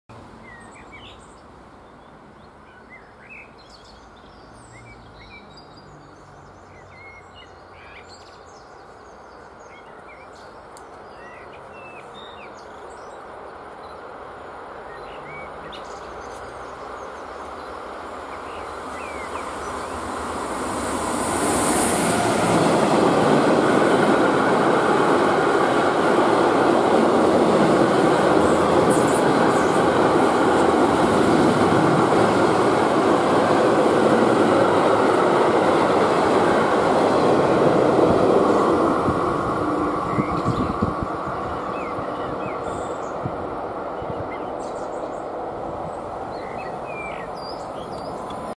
BR 185 203-7 DB Cargo
Durschfahrt  auf der strecke zwischen Bad Sassendorf-Soest   BR 185 DB Cargo mit Audizug.
auf der KBS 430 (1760).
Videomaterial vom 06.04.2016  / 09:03

Przelot na szlaku pomiędzy Bad Sassendorf i Soest lok.elektrycznej serii BR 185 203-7 DB Cargo ze składem autolawet Audi Autoexpress.
auf der KBS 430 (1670) .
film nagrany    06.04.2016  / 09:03